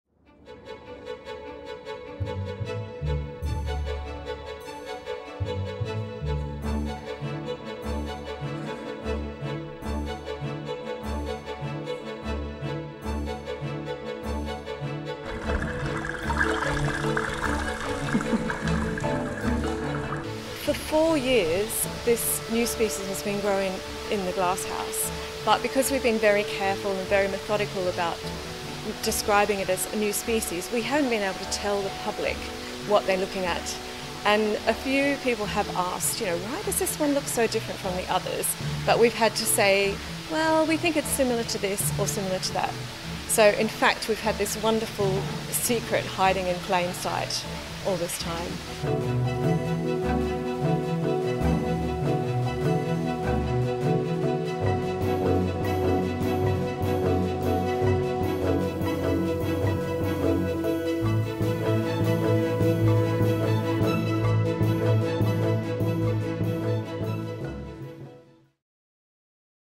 First new giant waterlily species discovered in London in over a century | AFP
The world's largest giant waterlily - and the first to be discovered in over a century - is unveiled at London's Kew Gardens, whose herbarium has been home to misidentified plant species for 177 years. The species was uncovered by a team in the UK and partners Bolivia (where the plant is native) using a blend of modern science, including DNA analysis and molecular technology, and the centuries-old practice of botanical drawings. It is the world's third giant waterlily.